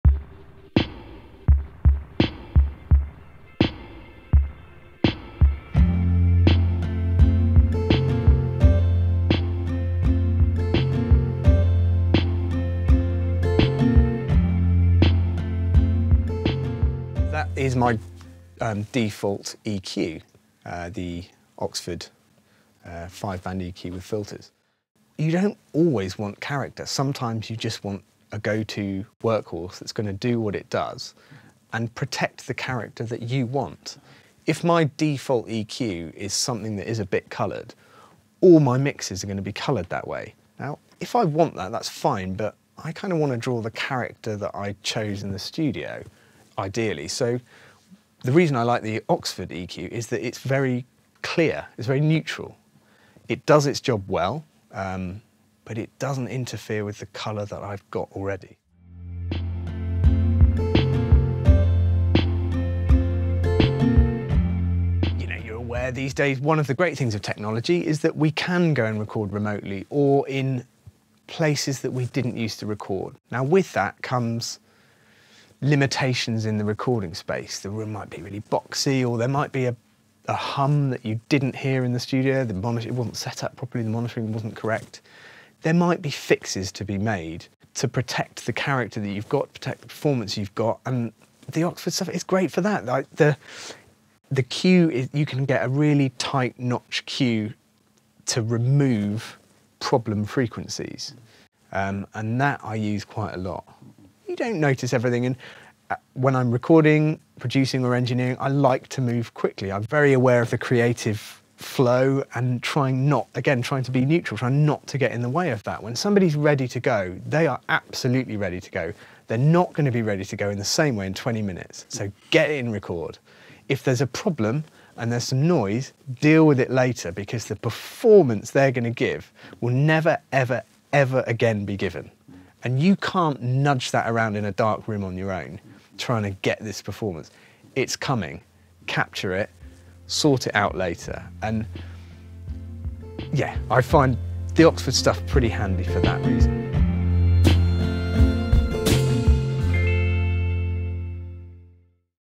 In The Studio with Matt Lawrence (Part 1)
Grammy-winning engineer, mixer and producer Matt Lawrence tells us why the Oxford EQ is invaluable to his process.

Massive thanks to Matt, all at Miloco's wonderful Pool Studios and, of course, the superb artists Tom Chaplin and Jones.